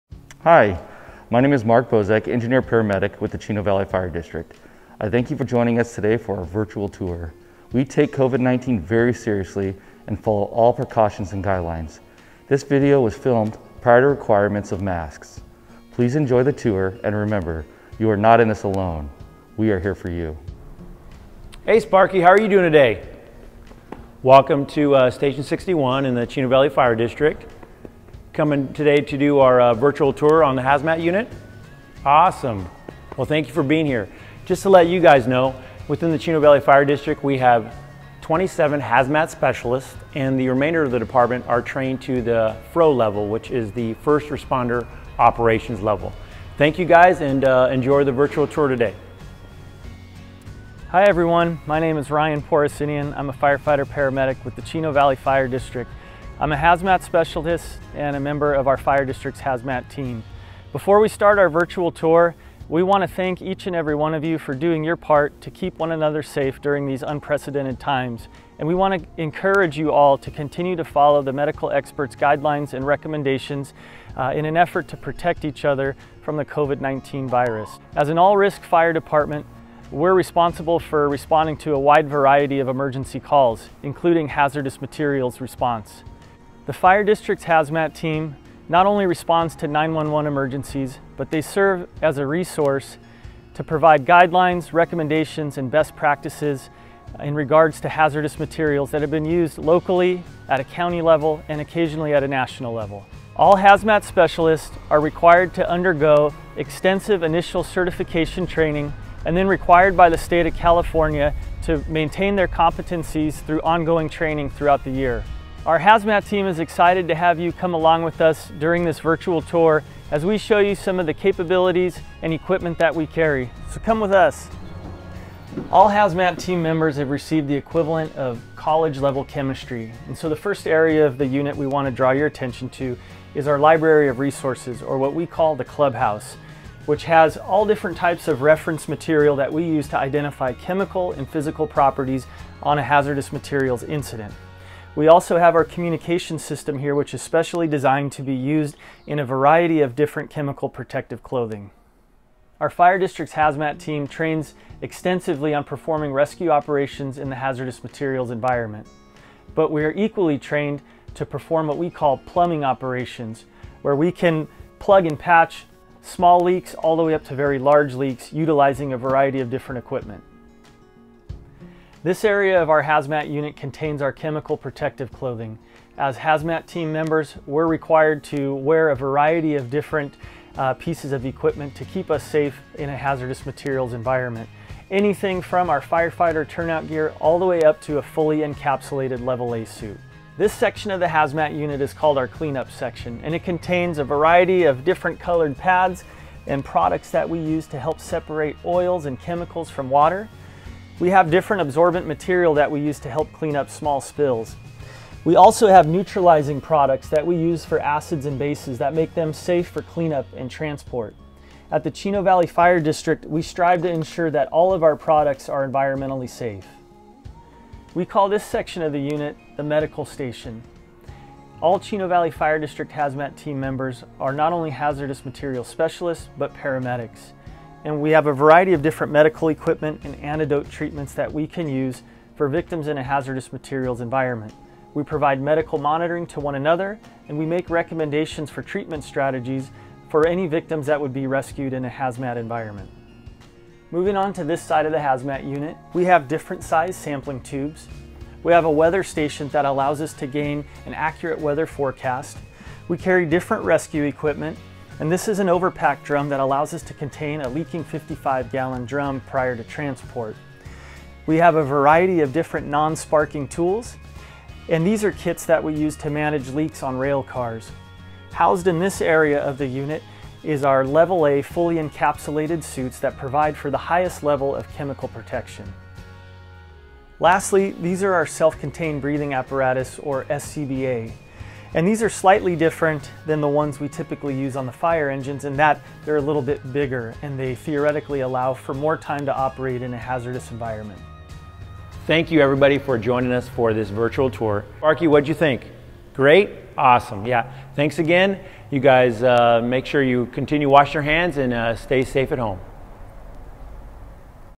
CVFD Hazardous Materials Unit Virtual Tour
Welcome to the Chino Valley Fire District's Hazardous Materials Unit virtual tour. In this virtual tour you will join our firefighters as they walk you through the various equipment and capabilities your Fire District has to ensure we are able to respond to any type of emergency, including those involving hazardous materials.  We truly are an all-risk fire department.